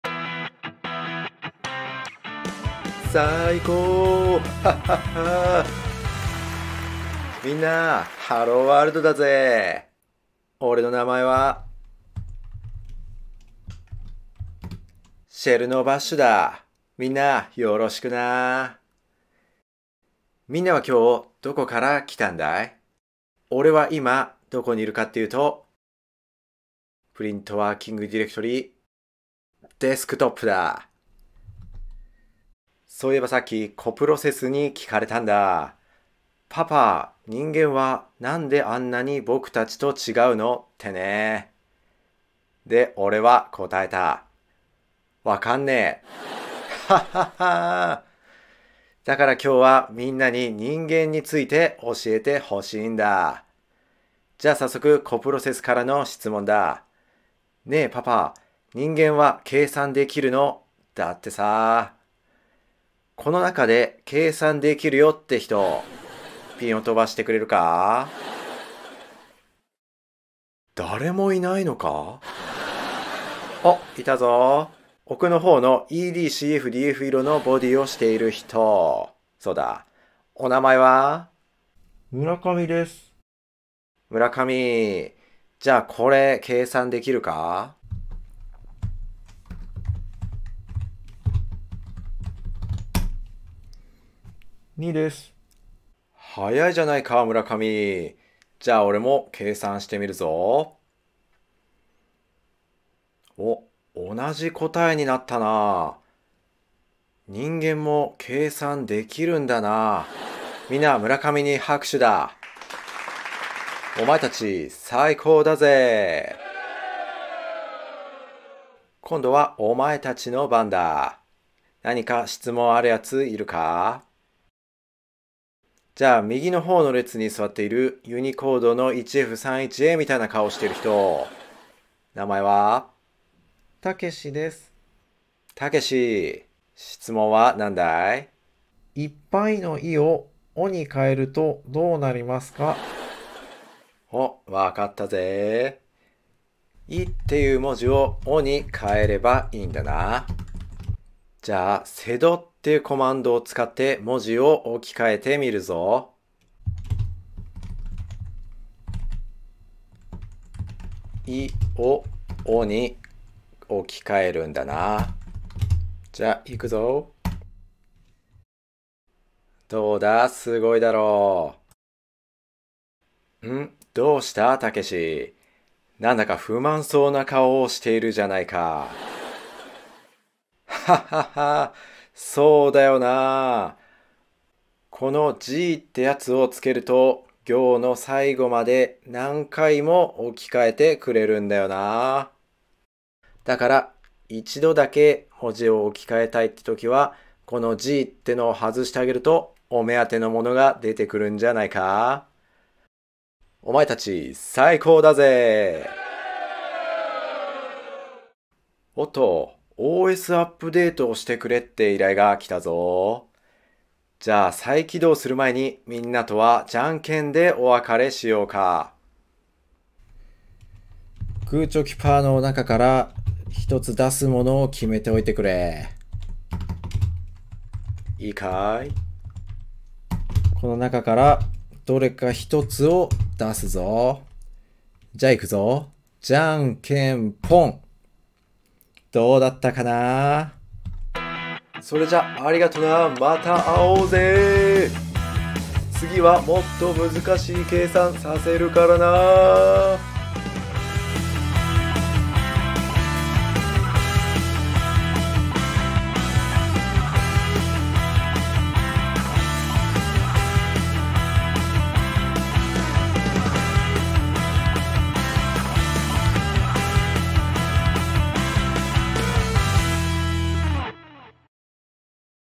【ターミナル・トーク】（echo/pwd/bc/sed/shuf）
ついでにシェルコマンドも覚えて帰ってくれ〜！
バッシュとお話できる人気アトラクション『ターミナル・トーク』です